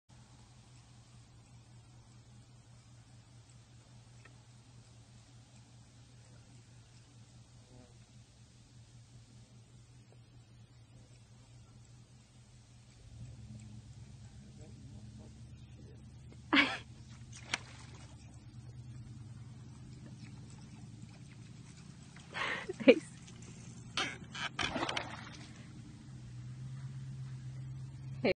Lizard is killing the topwater bite!!!
Gator in the cove would not leave us alone that evening.